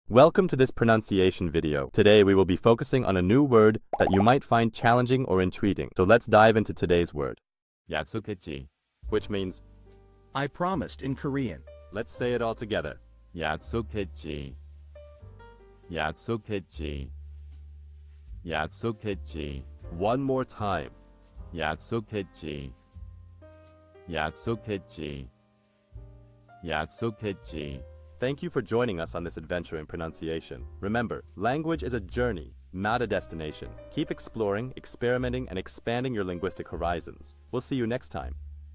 How to pronounce yaksokhaetji | 약속했지 (I promised in Korean)
Master the Pronunciation of 'yaksokhaetji | 약속했지' - which means : I promised in Korean 📝 with @PronunciationKorean 🎓🗣️ - [Your Guide to Korean]

Learn to pronounce 'yaksokhaetji' perfectly in Korean with our easy and engaging tutorial by @PronunciationKorean! 📖📽️ In this comprehensive video, we provide you with native speaker examples 🗣️👂 and phonetic breakdowns 🎵 to ensure you get the pronunciation just right.